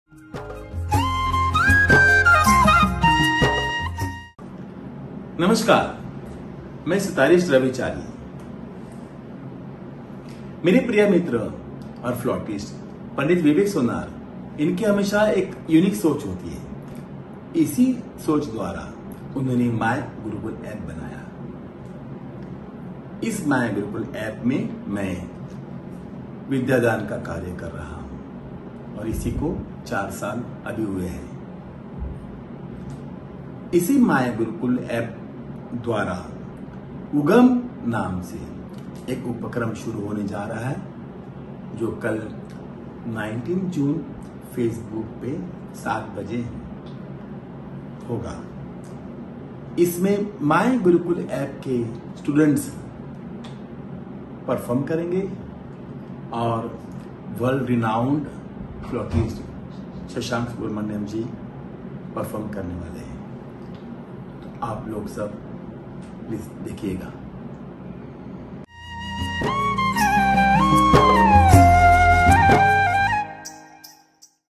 myGurukul’s Upcoming Online Event - "Ugam - Aspiration to Inspiration" | Pt. Ravi Chary
Pt. Ravi Chary expresses his thoughts about Ugam -“Aspiration to Inspiration”

A humble invitation to join the live event where myGurukul students & Vidwan Shri. Shashank Subramanyam will create a  blissful experience with their melodious flute recitals.

Date: 19th June 2021 at 7:00 pm IST | 2:30 pm BST | 9:00 am EDT